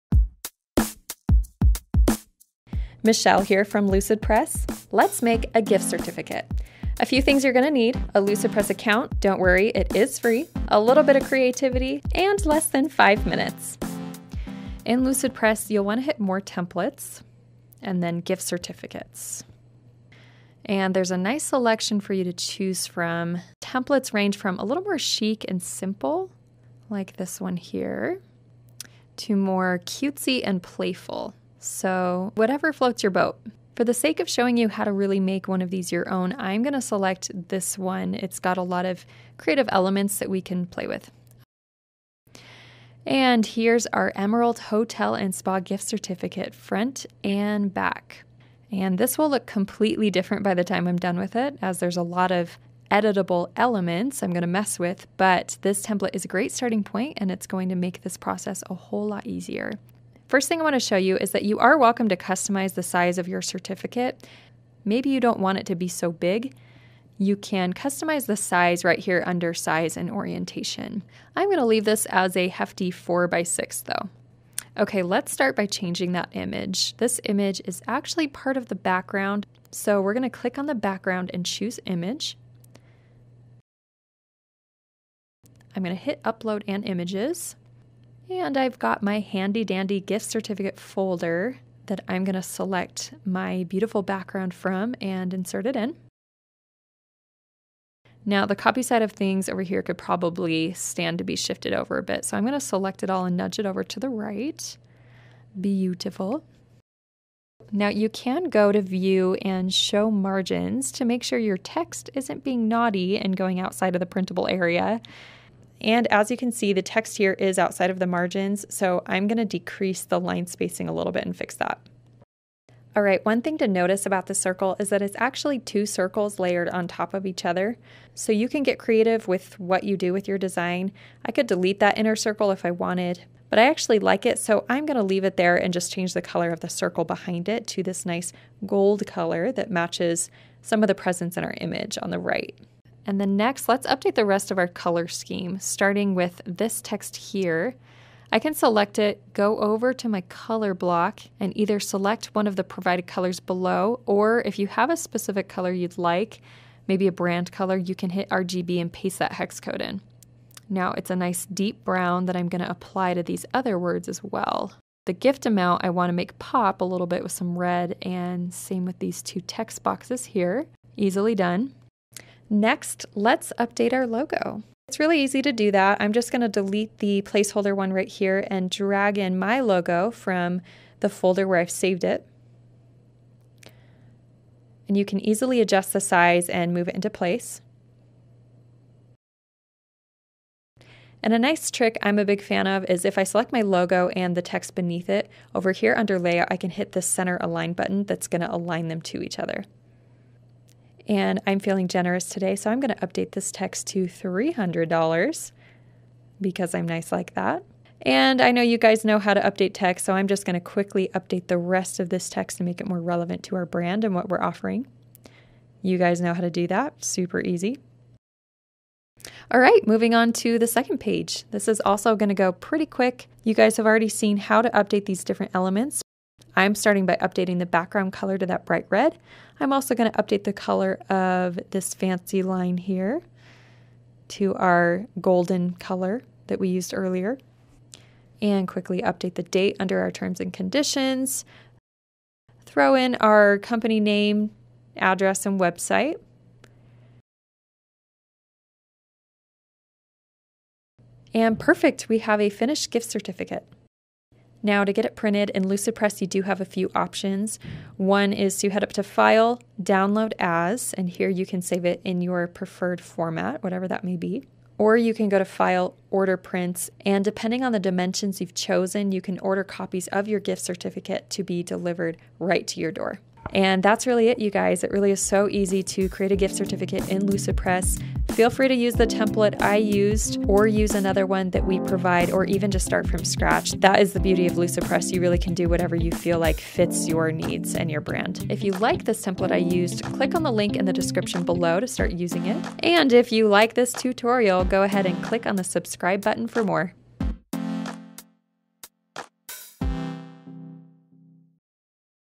How to make a gift certificate (free template included)
Learn how to make a gift certificate, no design experience necessary.

This tutorial walks you through the easiest way to design a gift certificate from beginning to end. Lucidpress templates provide a simple blueprint for creating the beautiful gift voucher design you have in mind.

This tutorial will walk you through:

- Customizing size/orientation
- Using margins
- Inserting images
- Getting creative with shapes
- Custom color selection
- Inserting logos
- Center-aligning
- Font selection & adjustment
- Downloading the final file
- Printing & shipping